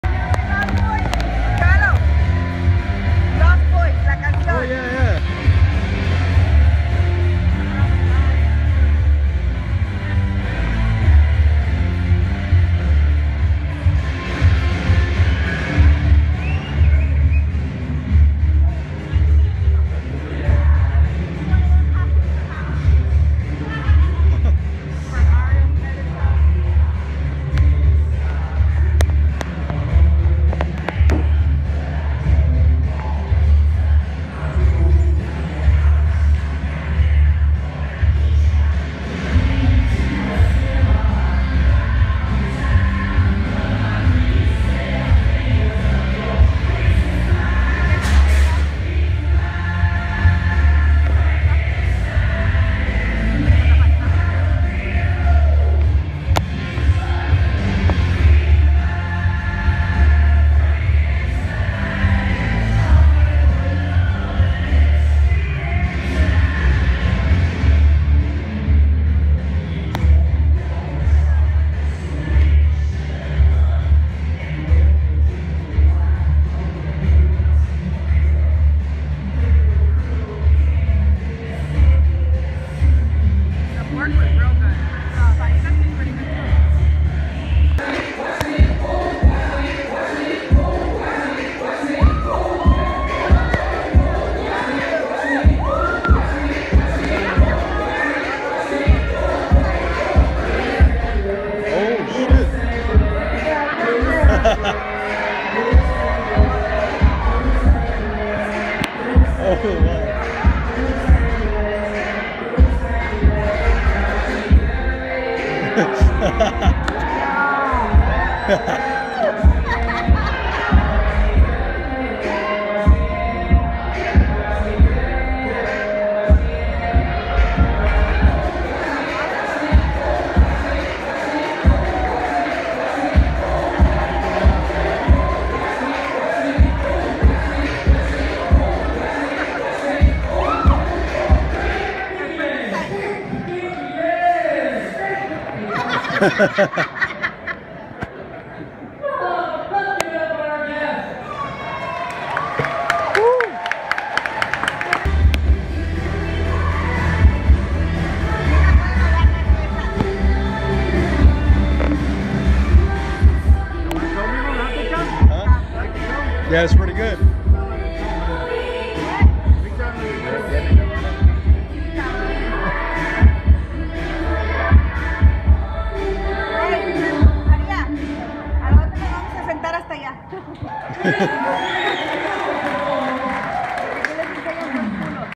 The Fiends show at Howl-O-Scream Busch Gardens 2015 during our fright feast!
My first year going to Howl-O-Scream at Busch gardens, Tampa Fl & man I had a blast last night! More content coming from howl-o-scream!